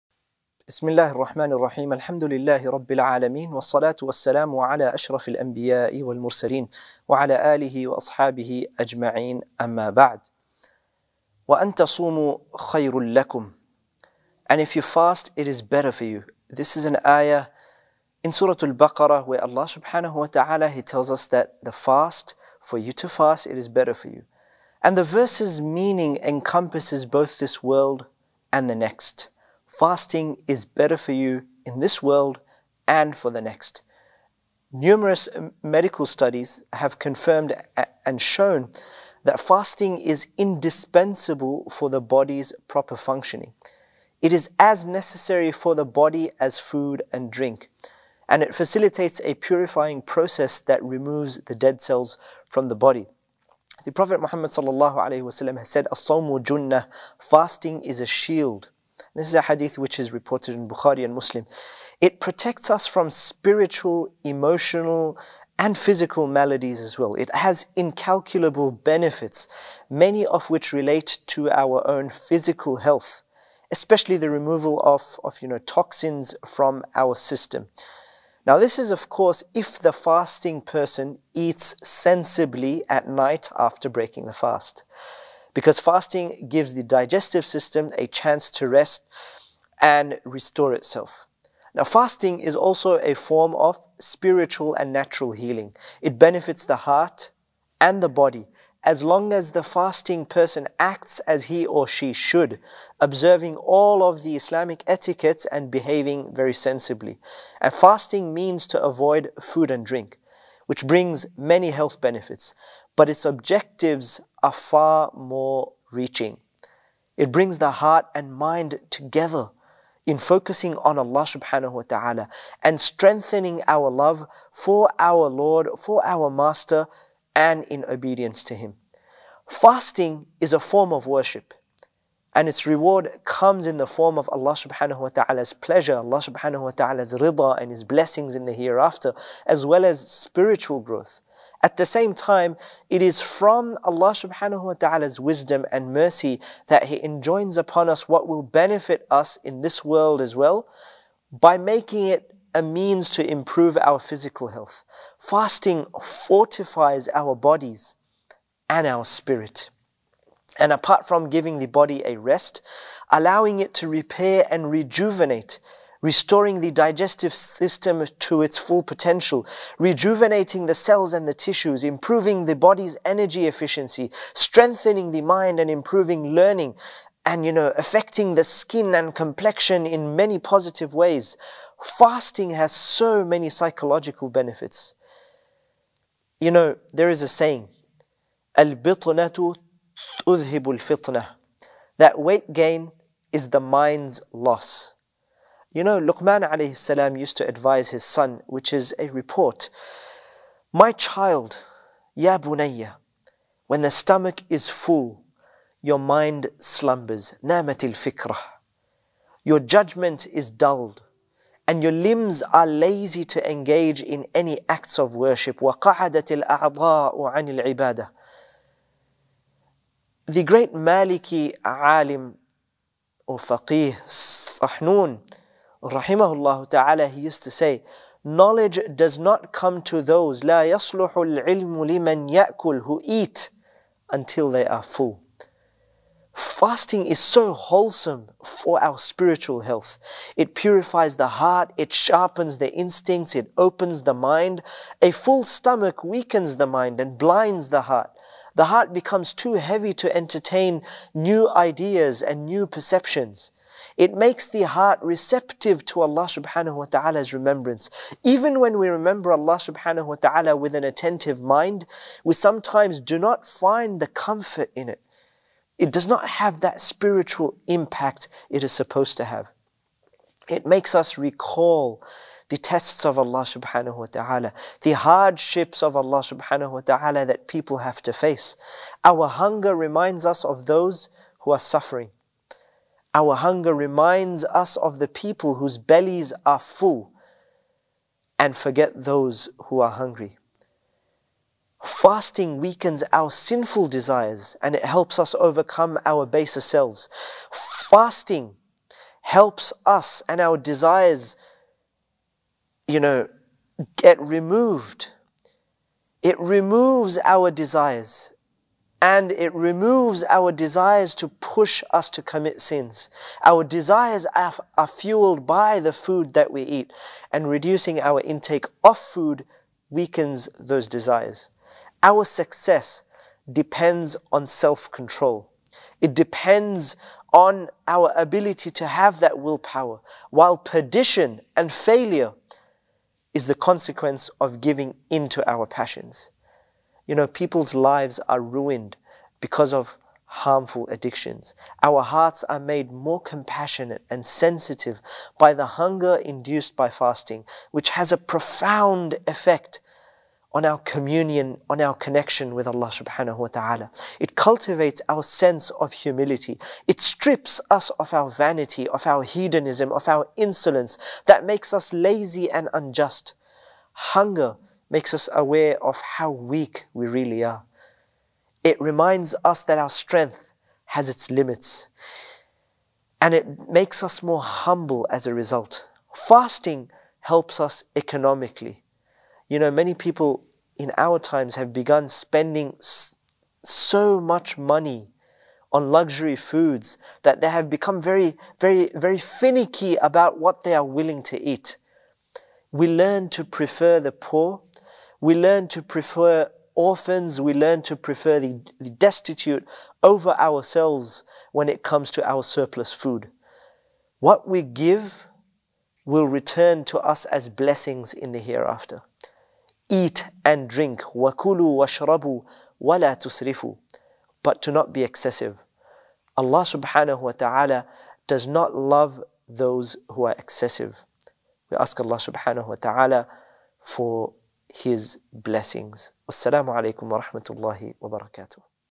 Day 7 | Daily Iftar Bytes: Fasting Healthy - Salmaan Parkar
Fasting is meant to flush out our physical & spiritual impurities. Rejuvenate yourself this Ramadan.